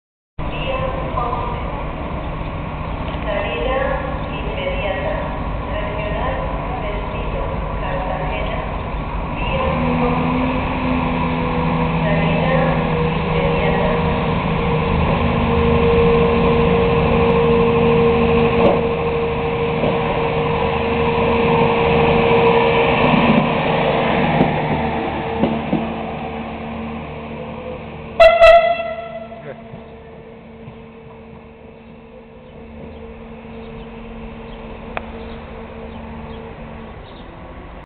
maniobra dresina en la estación de murcia el carmen 26-05-08
una dersina comienza una maniobra para cambiar de vía